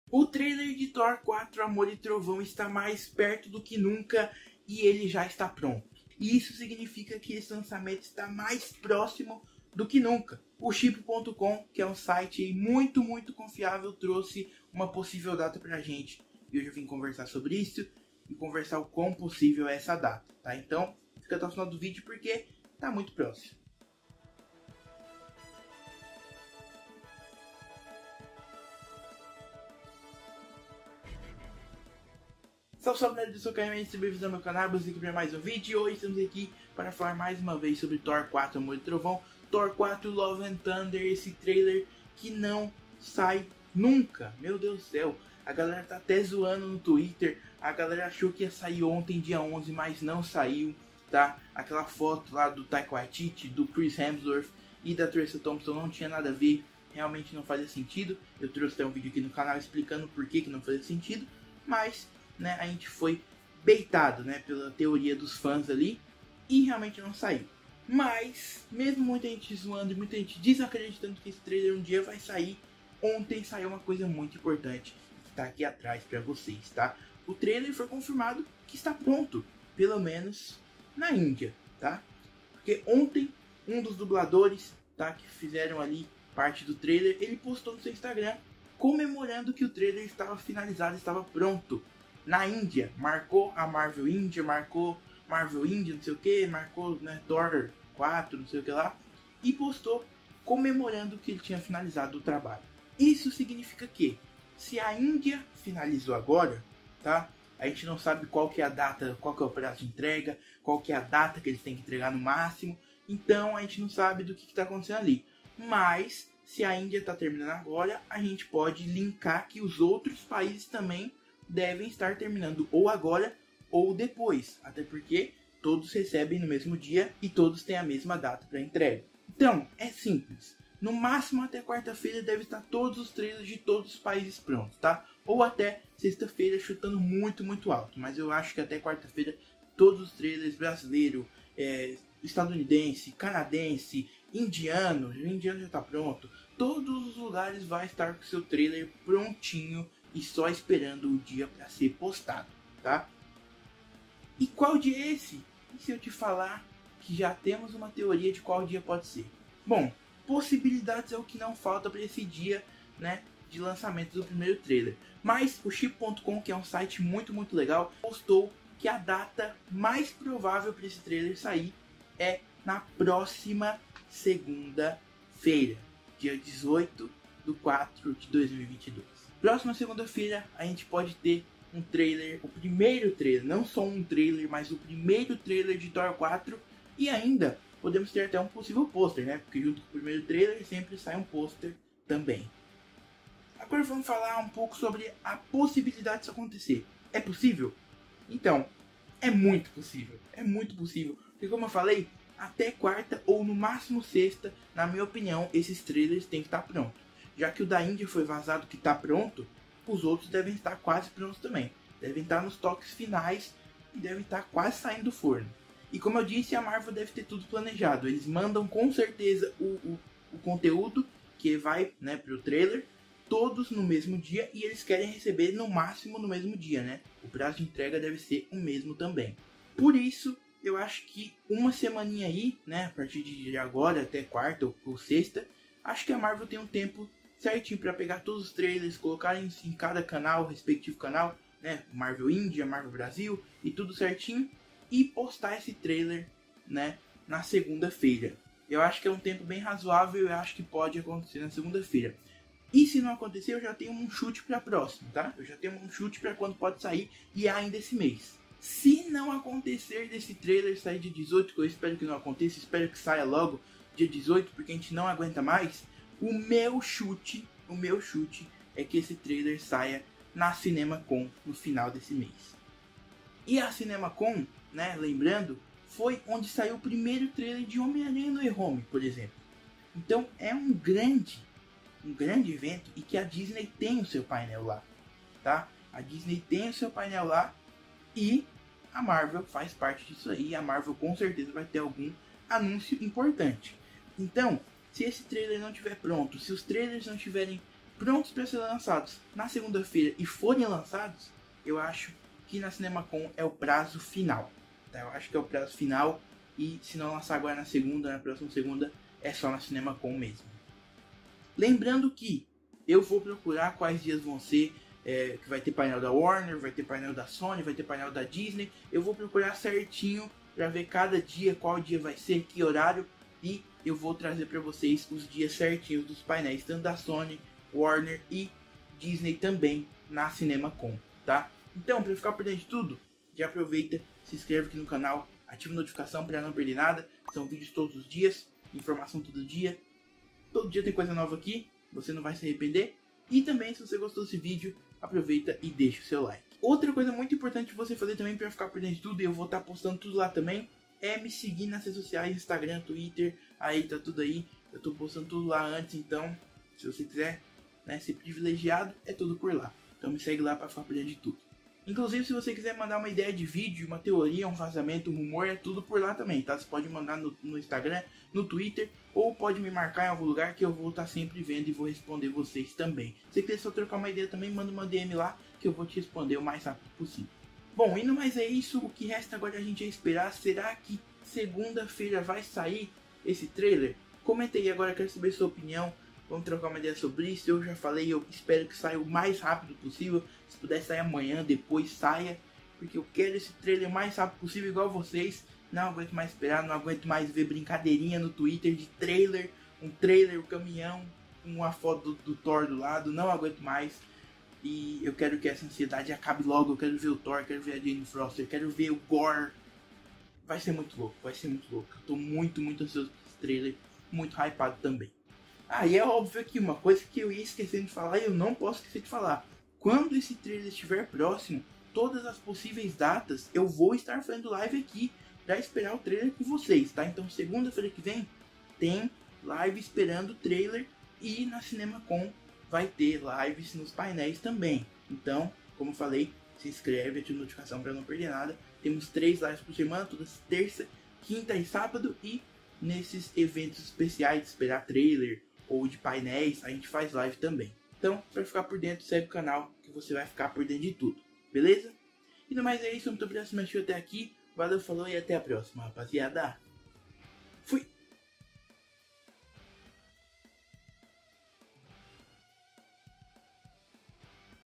PRIMEIRO TRAILER DE THOR 4 AMOR E TROVÃO ESTÁ PRONTO PARA SER LANÇADO!
Restam pouco menos de três meses para que Thor: Amor e Trovão, o 4º filme solo do herói da Marvel, chegue aos cinemas brasileiros, estreia que está marcada especificamente para o dia 7 de julho.

A impressão que fica é que falta muito mais tempo para o filme estrear, afinal a Marvel ainda não iniciou a campanha de divulgação de Thor 4, ainda que falte tão pouco para a estreia. O filme até quebrou um recorde negativo na história do MCU, como trailer que mais demorou para lançar.

Por enquanto os fãs ainda aguardam pelo primeiro trailer do filme, que deve dar uma prévia do grande evento que Amor e Trovão promete ser, contando com a presença dos Guardiões da Galáxia e o retorno da Jane Foster (Natalie Portman), que irá se transformar na Poderosa Thor.

Mas hoje o dia começou com o dublador indiano do Thor revelando que o trailer já está pronto, e logo depois surgiram mais informações indicando exatamente quando iremos ver o primeiro trailer de Thor 4.

O site Chippu, que recentemente revelou a data de estreia de She-Hulk e informações sobre Pantera Negra 2, afirmou que o trailer será lançado daqui há uma semana, no dia 18, próxima segunda-feira.
VÍDEOS RELACIONADOS